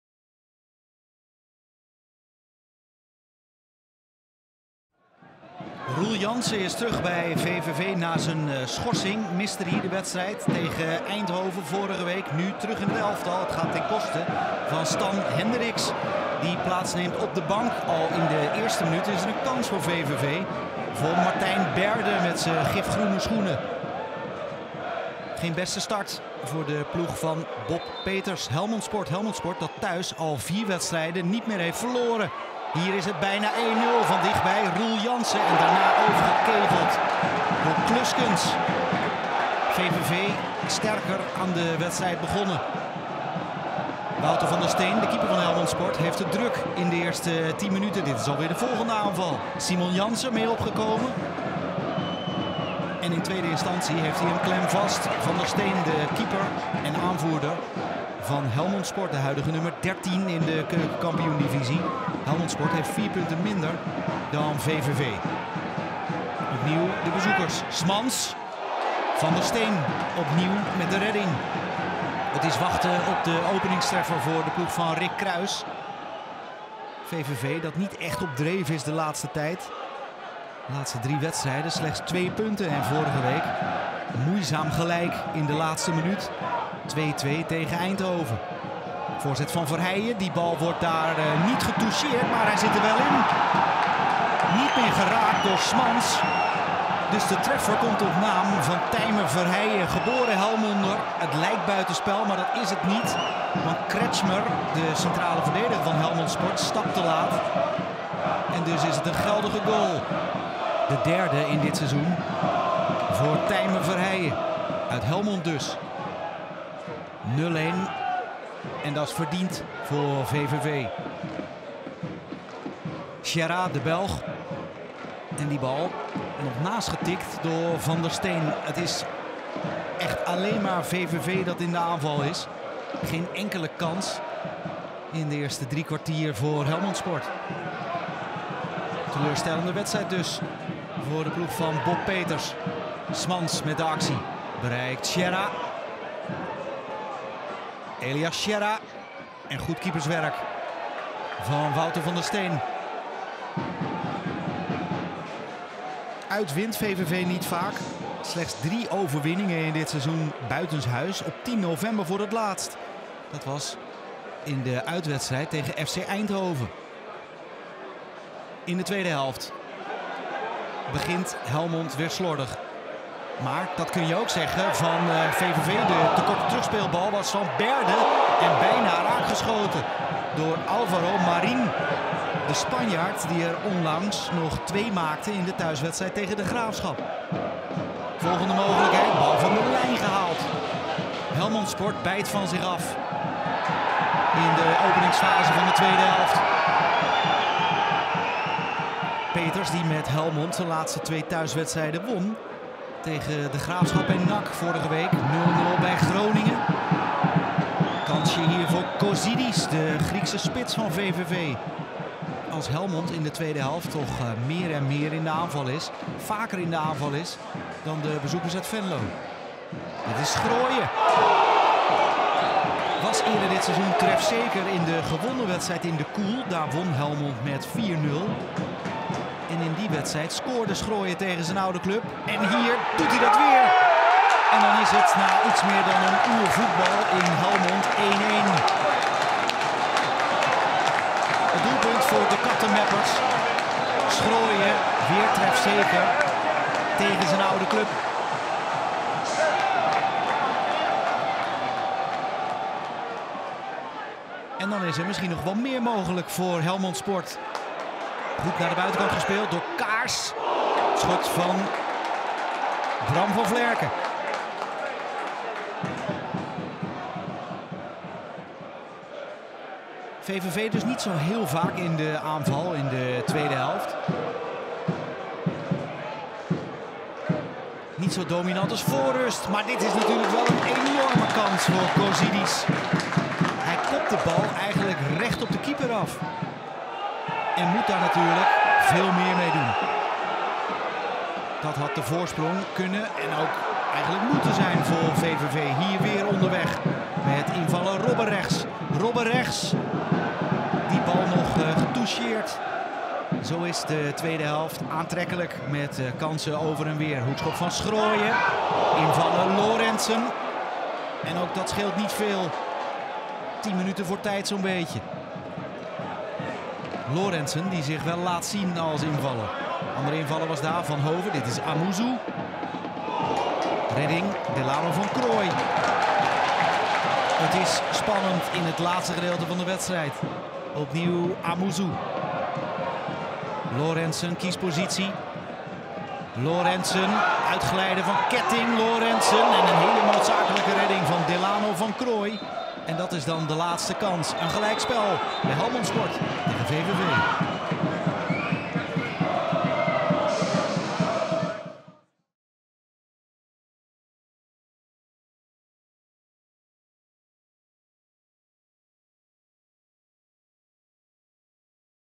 HIGHLIGHTS | Helmond Sport blijft steken op gelijkspel tegen VVV 🙁
Samenvatting van Helmond Sport - VVV (1-1)